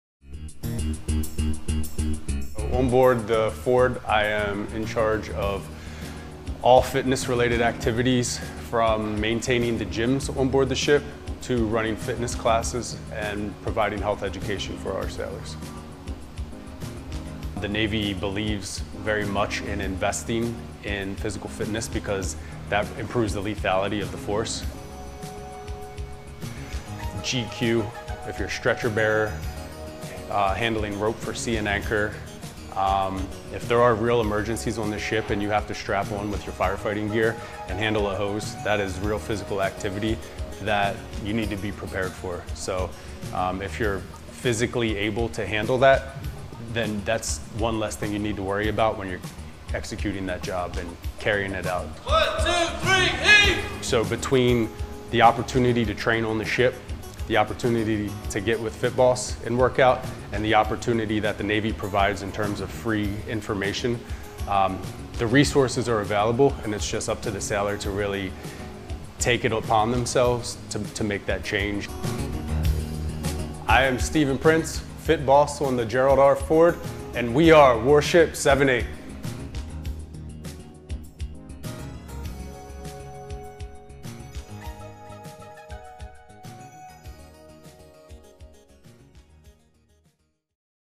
FITBOSS We Are Warship 78 (2019) 🇺🇸
🔴 [Author]:
Video by Petty Officer 2nd Class Ruben Reed

🔴 [Location]:
Newport News, Va, United States

🔴 [Description]:
NEWPORT NEWS, Va. (Sept. 18, 2019) USS Gerald R. Ford's (CVN 78) fitness coordinator, also known as FITBOSS, is highlighted as this week's We Are Warship78 feature. Ford is currently undergoing its post-shakedown availability at Huntington Ingalls Industries-Newport News Shipbuilding. (U.S. Navy video by Mass Communication Specialist 2nd Class Ruben Reed)

🔴 [Tags]:
PSA, USS Gerald R. Ford (CVN 78), Navy, MWR, Sailors, US Navy, training, GRF, FITBOSS, First In Class, Warship78

🔴 [VIRIN]:
190918-N-QI093-1001